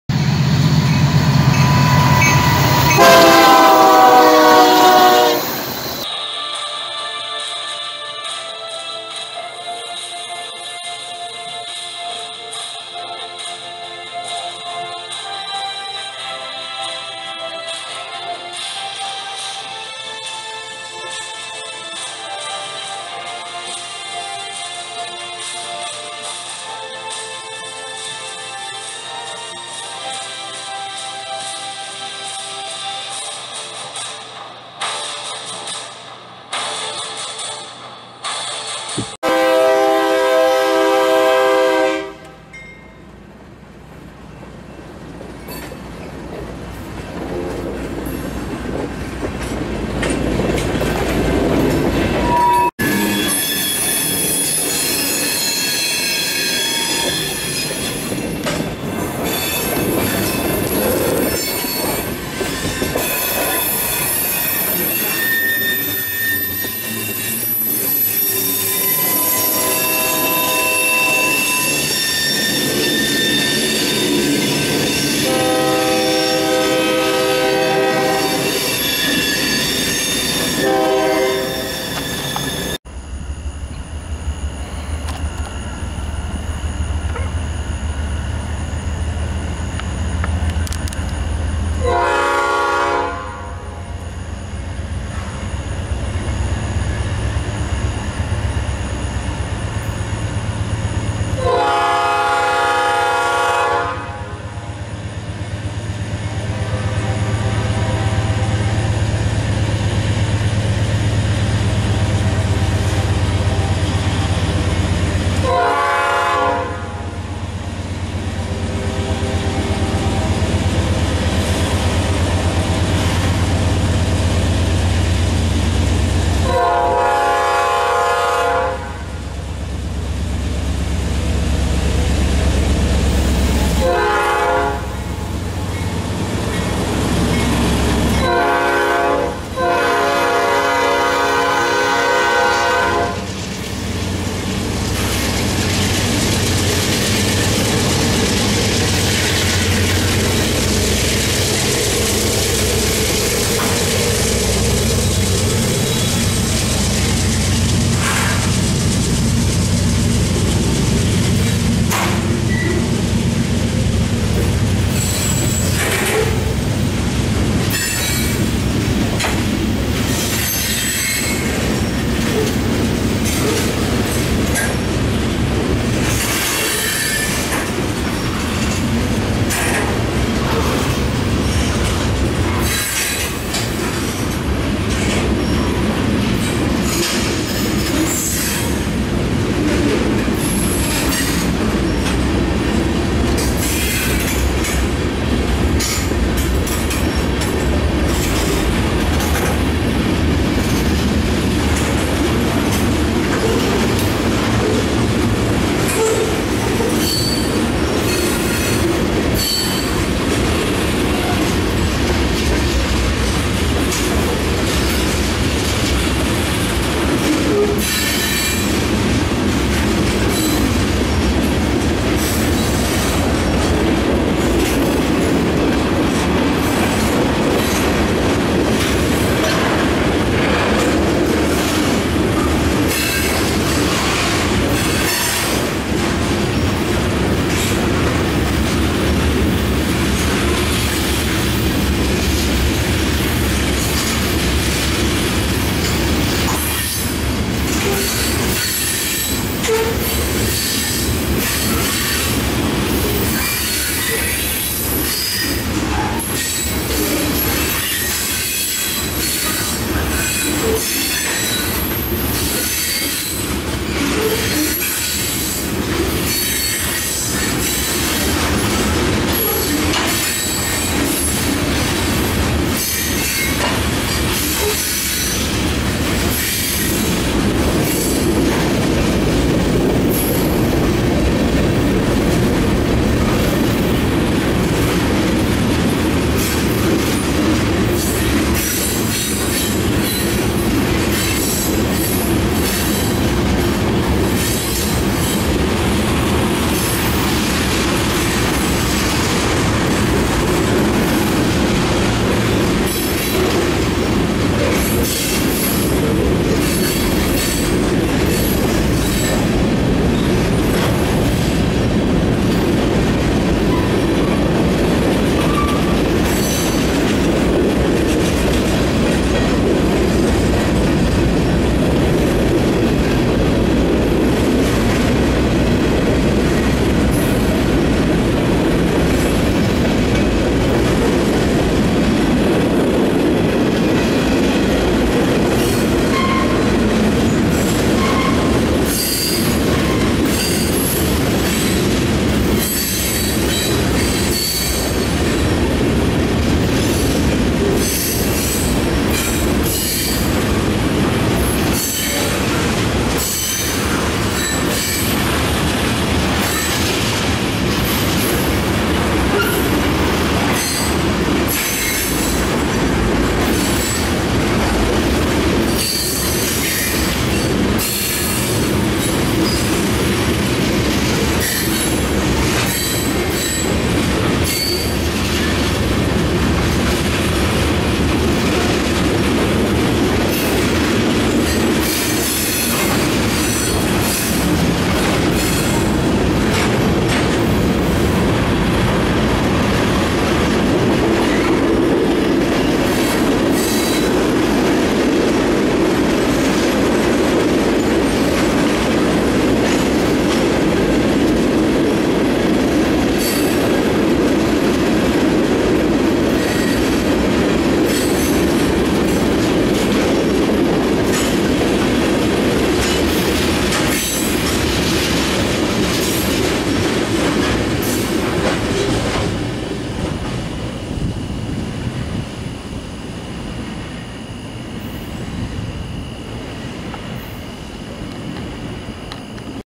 BNSF Local & UP Manifest at Crockett Street
I was able to spend an evening in Beaumont, Texas railfanning before work. While I was at Crockett Street, I was able to catch a west bound UP manifest freight train with an NS leader and a short BNSF local.